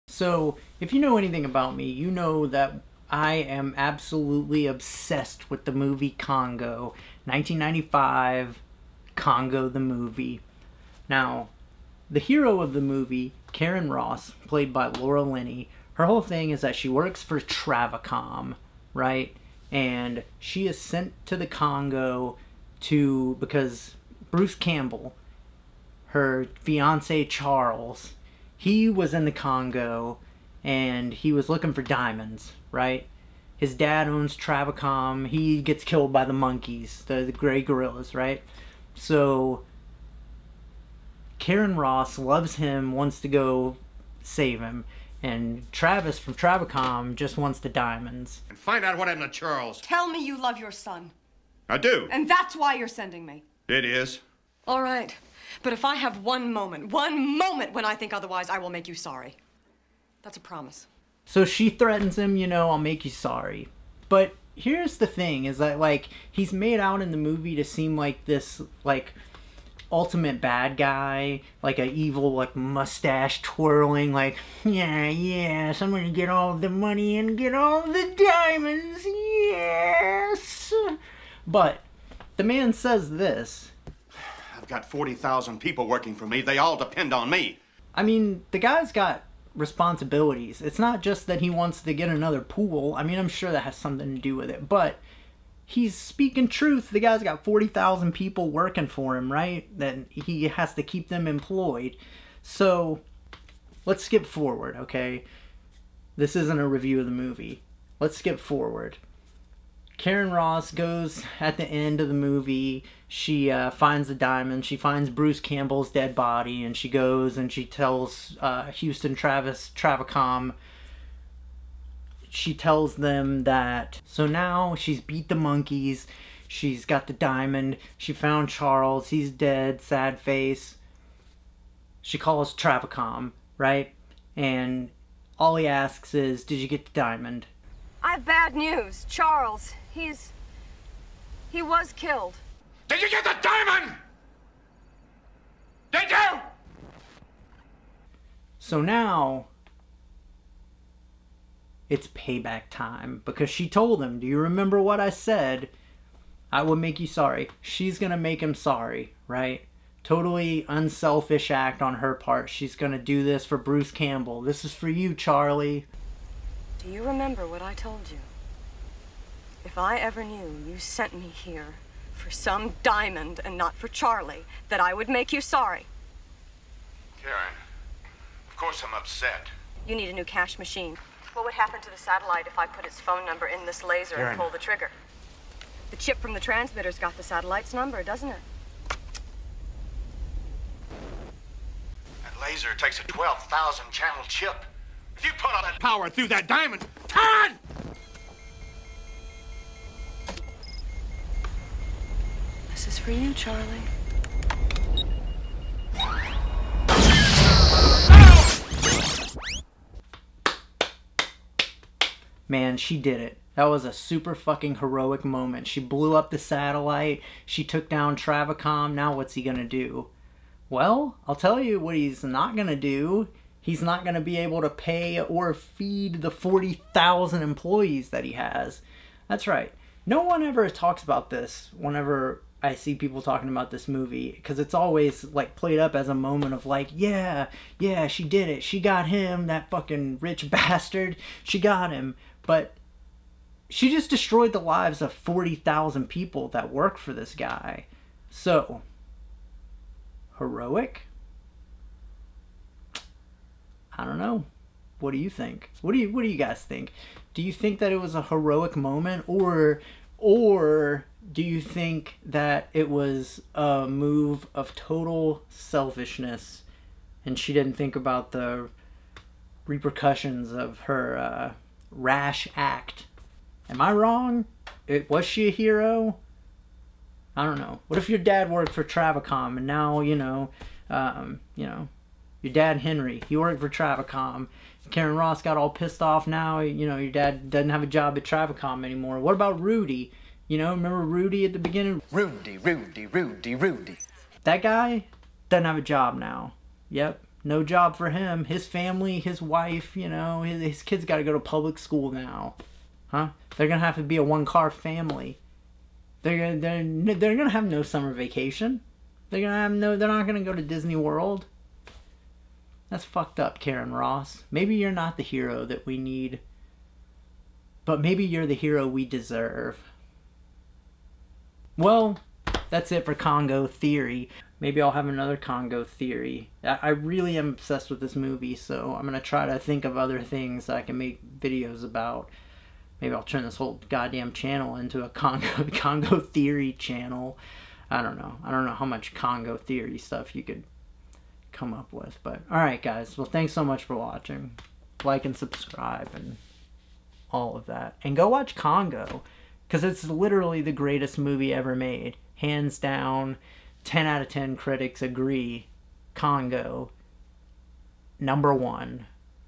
How Karen Ross Destroyed 40,000 Lives - Congo Theory
Karen Ross, the hero of Congo (1995) really goes the extra mile for her revenge! Let's take a deep dive!